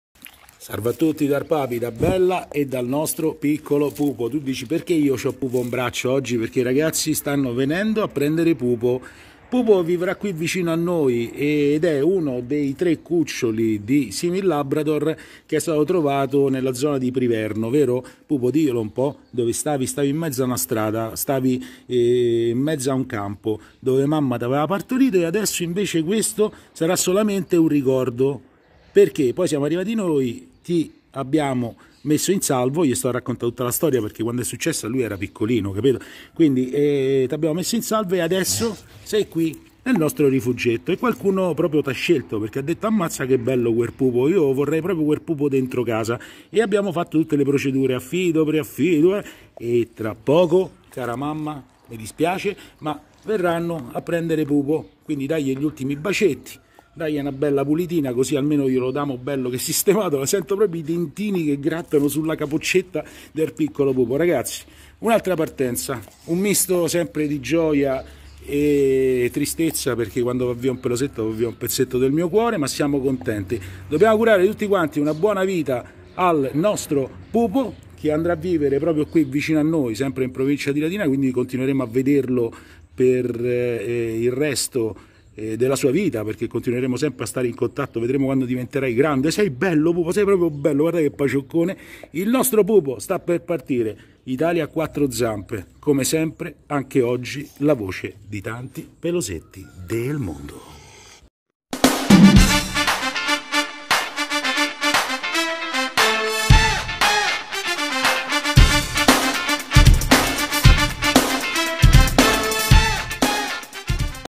💛Italia a 4 zampe ErPapy💛
Priverno LT
Ci salutiamo così con
❤PUPO❤...presente anche mamma
💛BELLA 💛 che lo pulisce perché lei è una mamma perfetta...che cerca ancora casa...Labrador di circa 3 anni dolcissima.. bellissima...semplicemente speciale...abbandonata incinta...intanto Buona Vita a 😍PUPO😍